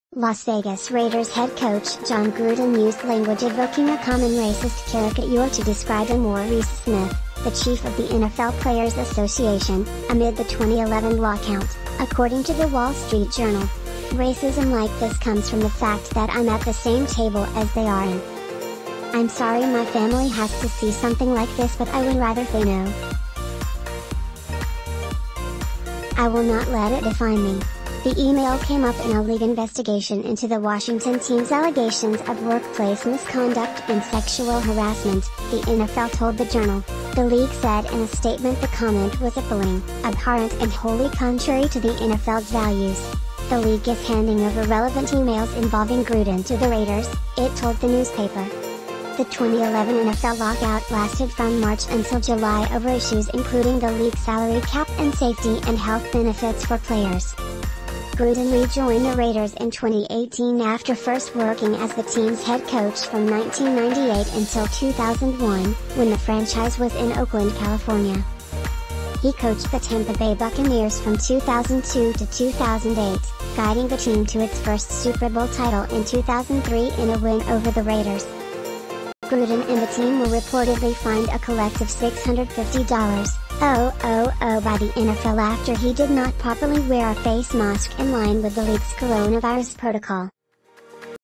Raiders Coach Reportedly Made Racist Comments About NFL Players Union
Las Vegas Raiders head coach Jon Gruden used language evoking a common racist caricature to describe DeMaurice Smith, the chief of the NFL Players Association, amid the 2011 lockout, according to the Wall Street Journal.“Racism like this comes from the fact that I’m at the same table as they are and they don’t think someone who looks like me belongs,” DeMaurice said. “I’m sorry my family has to see something like this but I would rather they know. I will not let it define me.”The email came up in a league investigation into the Washington Team’s allegations of workplace misconduct and sexual harassment, the NFL told the Journal. The league said in a statement the comment was “appalling, abhorrent and wholly contrary to the NFL’s values.” The league is handing over relevant emails involving Gruden to the Raiders, it told the newspaper. The 2011 NFL lockout lasted from March until July over issues including the league salary cap and safety and health benefits for players. Gruden rejoined the Raiders in 2018 after first working as the team’s head coach from 1998 until 2001, when the franchise was in Oakland, California. He coached the Tampa Bay Buccaneers from 2002 to 2008, guiding the team to its first Super Bowl title in 2003 in a win over the Raiders. Gruden and the team were reportedly fined a collective $650,000 by the NFL after he did not properly wear a face mask in line with the league’s coronavirus protocol. Jon Gruden Used Racial Trope to Describe NFLPA Chief DeMaurice Smith in 2011 Email (Wall Street Journal)NFL Hits Washington Team With $10 Million Fine For Culture Of Sexual Harassment (Forbes)